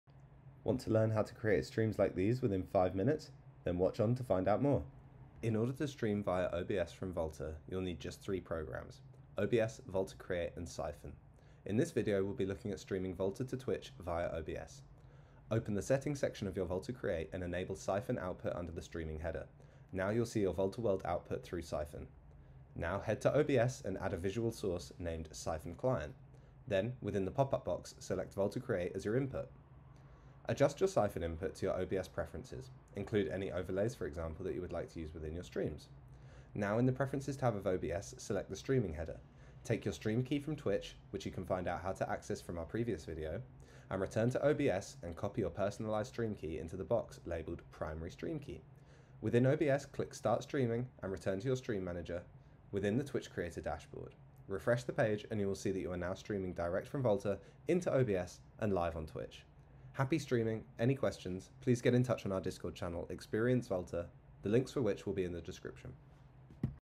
Top tip to create and design custom 3D visuals for your next Twitch stream!
Did you know that Volta Create can integrate seamlessly with your existing Twitch stream set-up? Either stream direct from Volta, or watch on to learn how to output your Volta Create performance via OBS.

With OBS you can continue to use your own custom assets, maintain maximum control over your stream, all whilst maximising engagement with your viewers!

Ableton user? Make sure you download our Max for Live Device